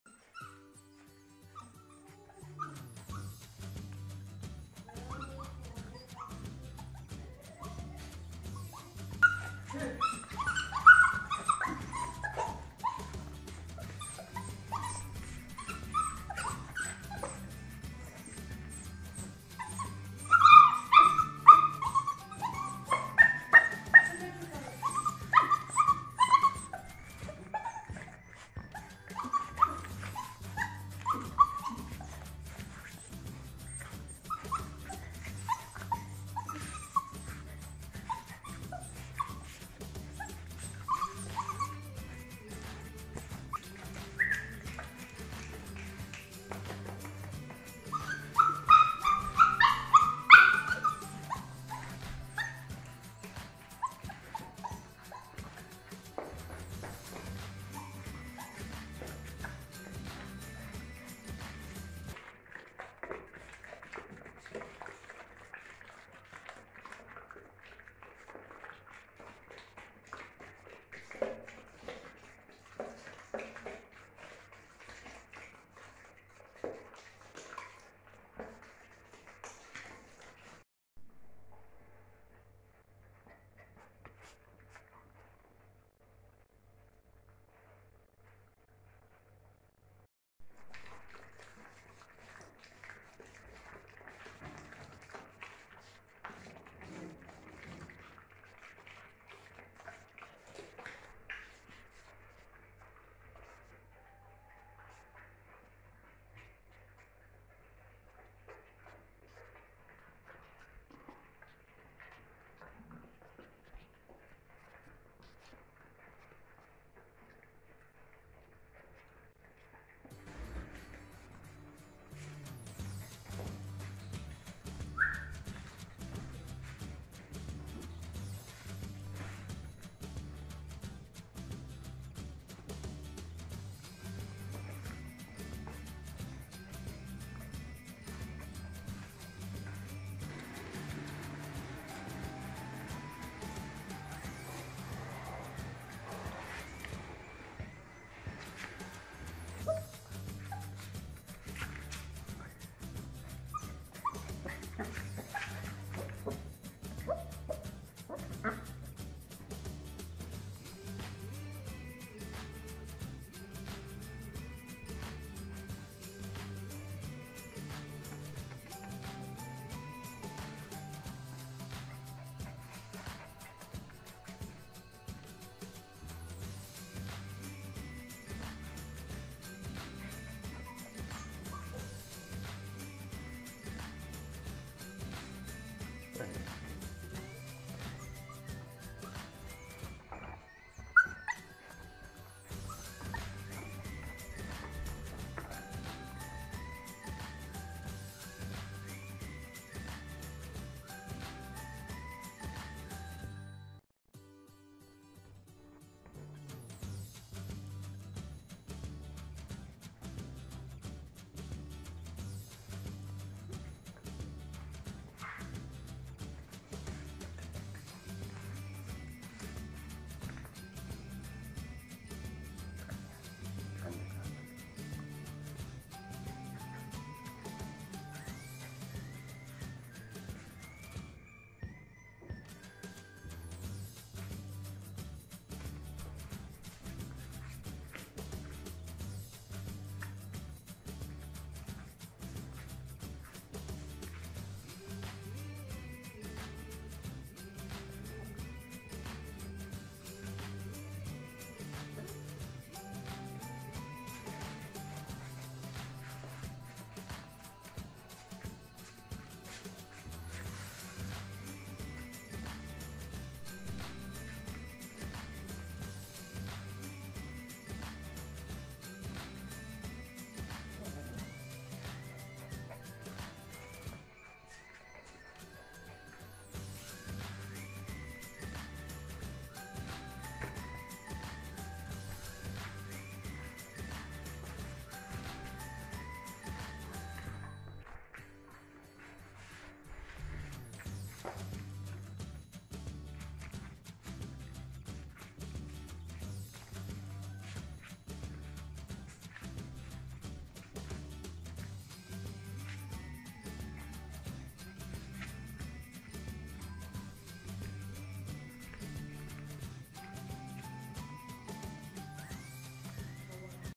Friendly puppy lost in the open (part 3)
The new puppy has a family of puppy friends, she is now very happy and healthy